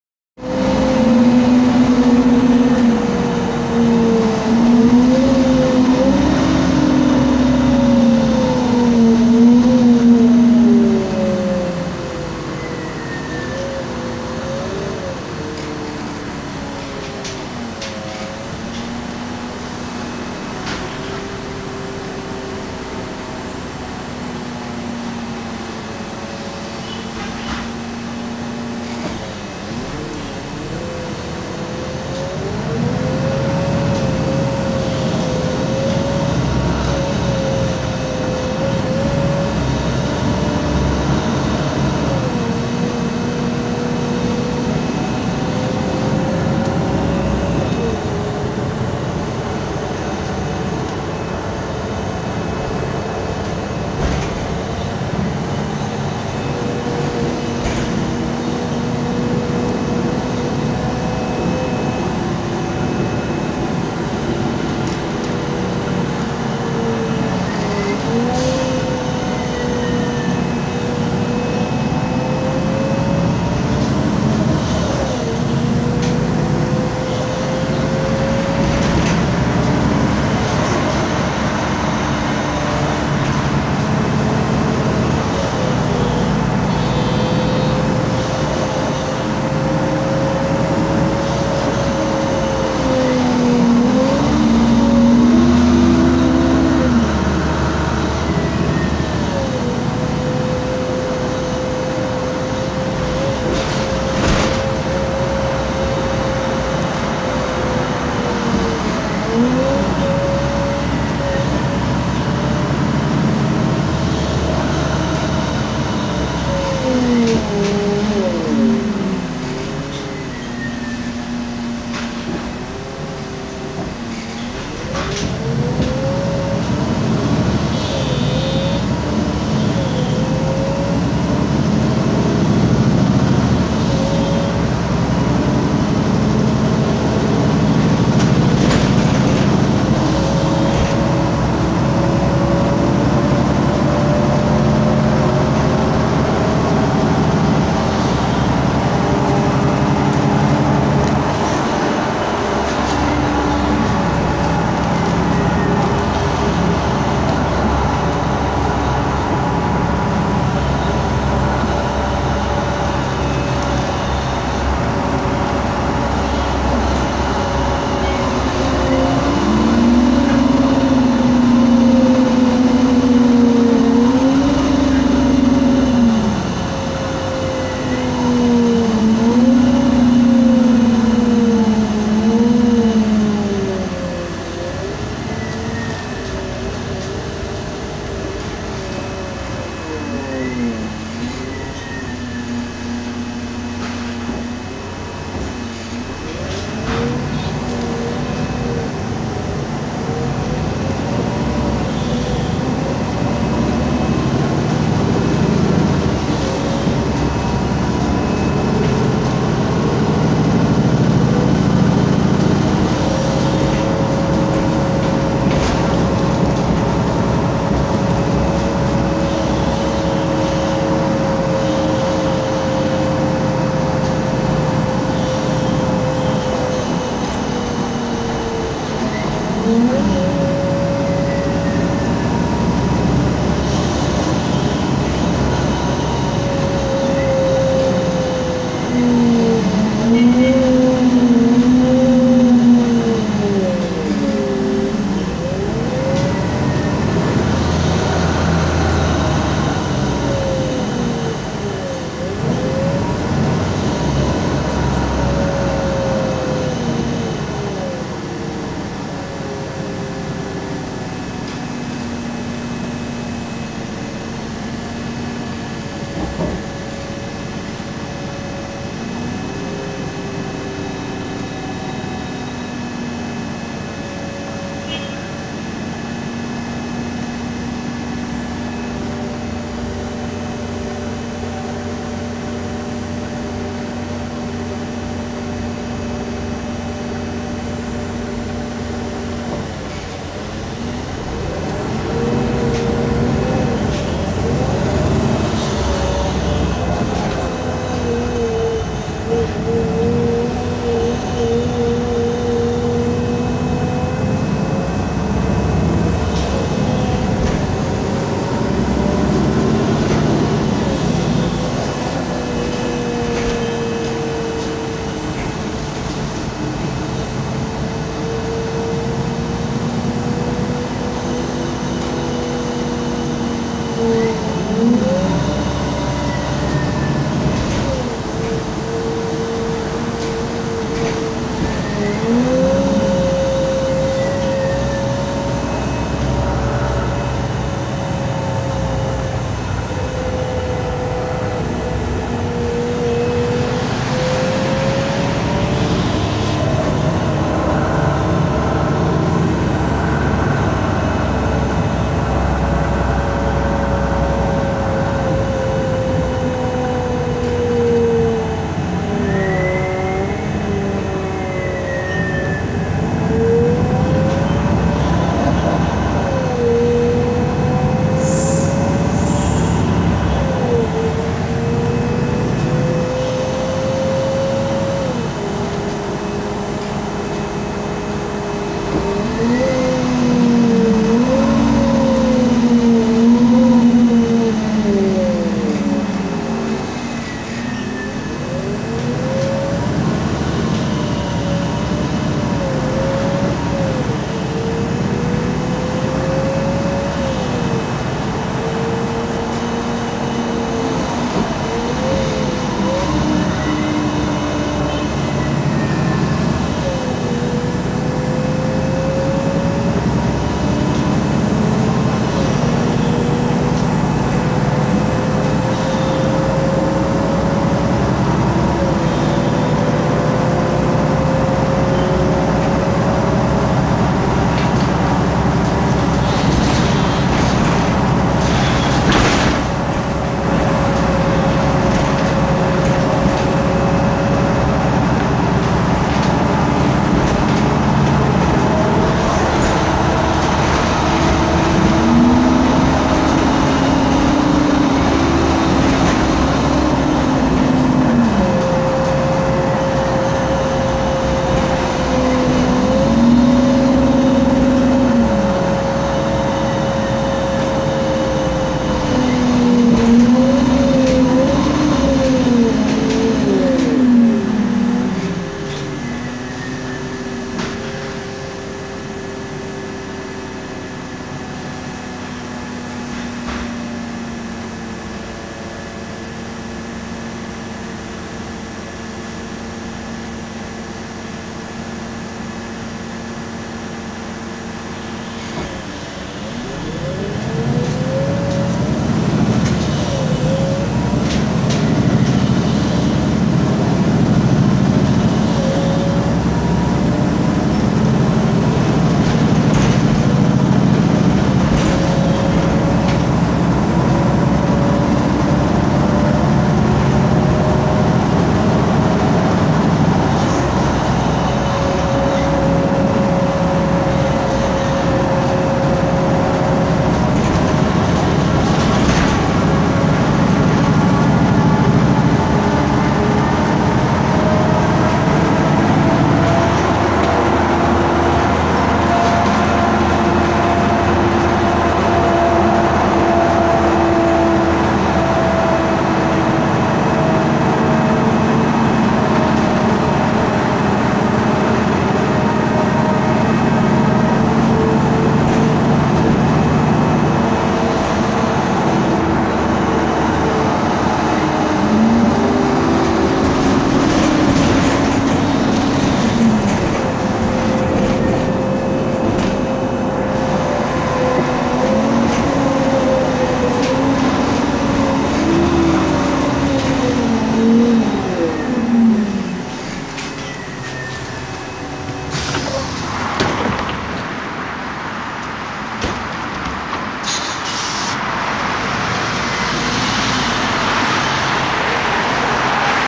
Man Lion's City g akşam yolculuğu Hat 408 Gülpınar Güneşevler birlik mahallesi kod numara 13-404
Teknik Özellikler Motor ManDe2876Luh02 EEV şanzıman ZF Ecolife 6AP1400B 228 KW 310 Beygir gücü.Yakit cng Metre uzunluğu 18.model yılı 2013 üretim yeri Ankara Man Fabrikası hizmete giriş Ocak 2013 iyi seyirler